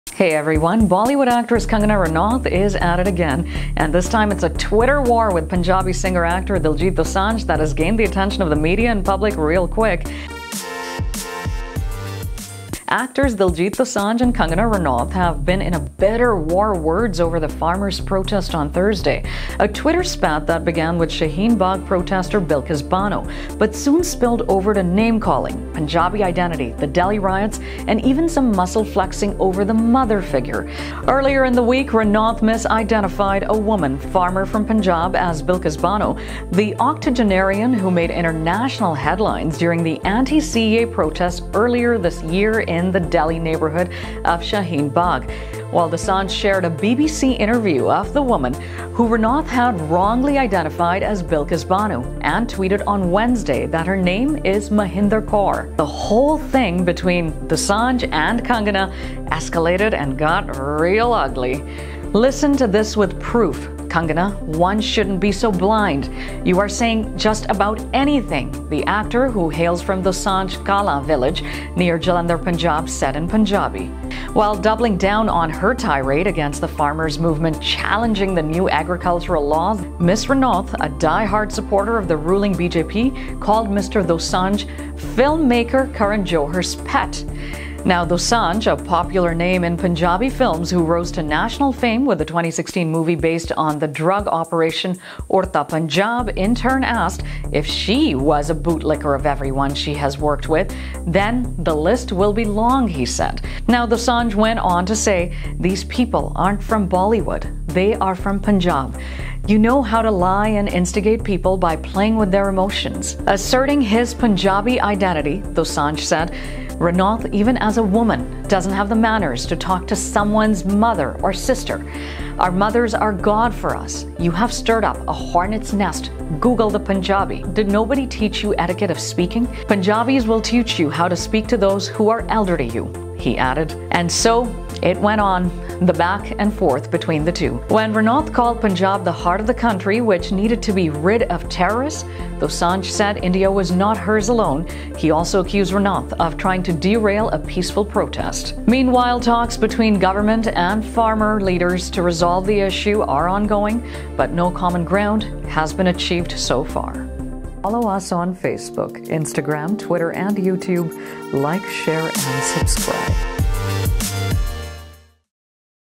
Diljit Dosanjh Blasts Kangana Ranaut on Twitter FarmersProtest | Tweets
KanganaRanaut and DiljitDosanjh Twitter Spat on Punjabi Identity values has hit the internet like a storm!

Bollywood actress Kangana Ranaut  is back in the spotlight again in relation to the farmers protests and this time has gone head to head with Punjabi singer actor Diljit Dosanjh in a bitter Twitter spat.